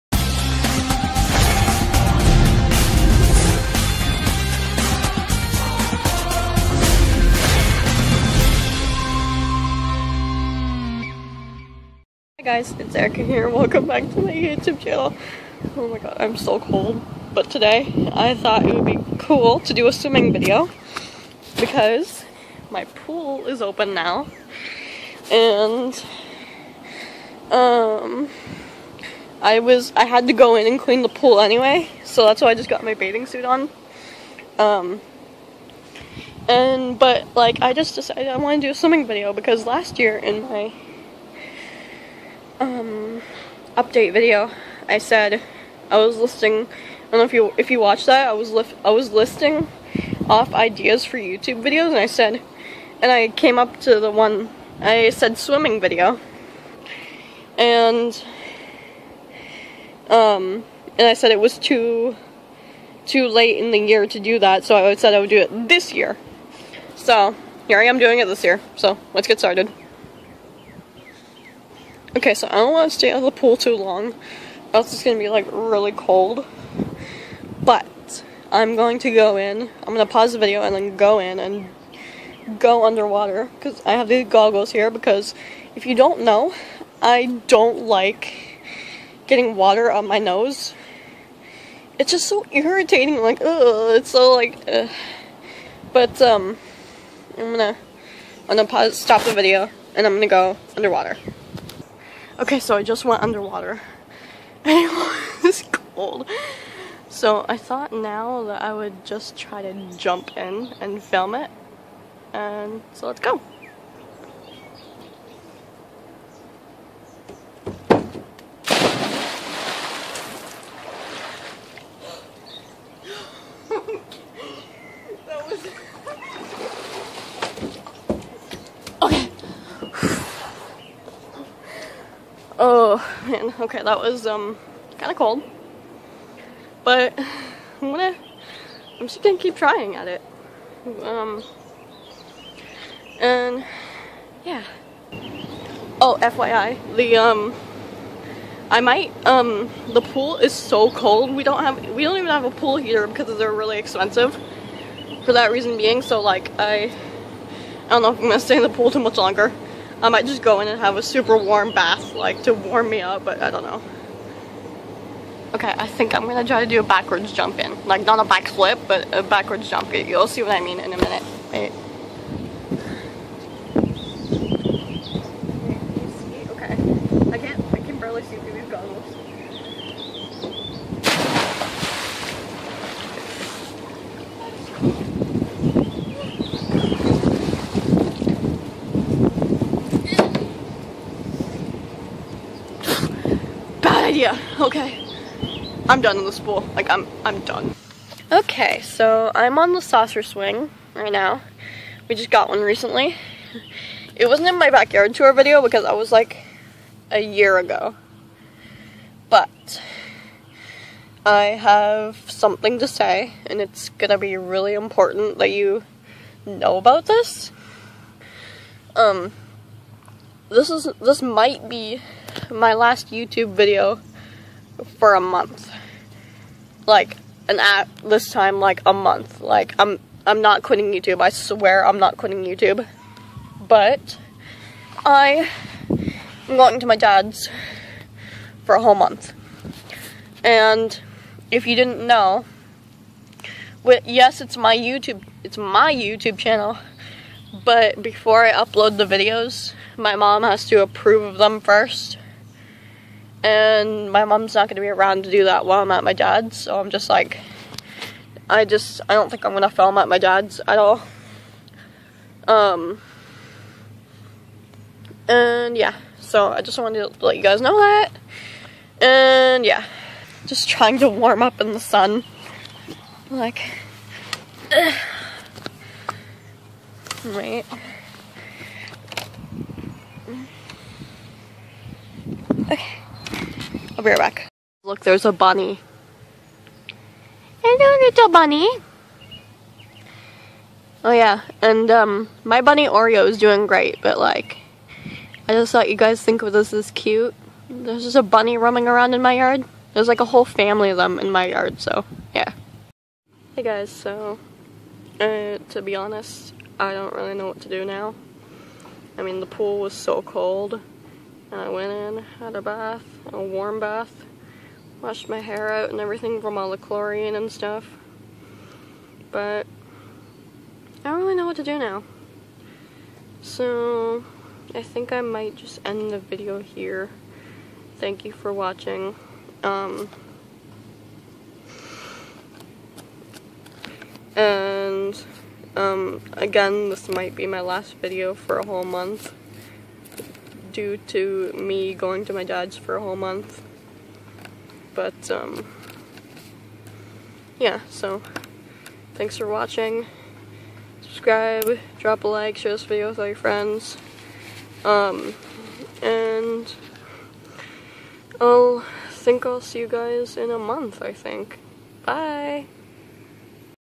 Swimming In A Cold Pool! *BAD IDEA*
So I decided to make a swimming video today but it didn't go well because it was so cold and as I said already this might be my last YouTube video for a month so there will be no videos at all in July. Unless I film another video before June 30 but I don't know if I will. I love you guys so much. If I don't post again then I will see you all in August. Thanks, Erica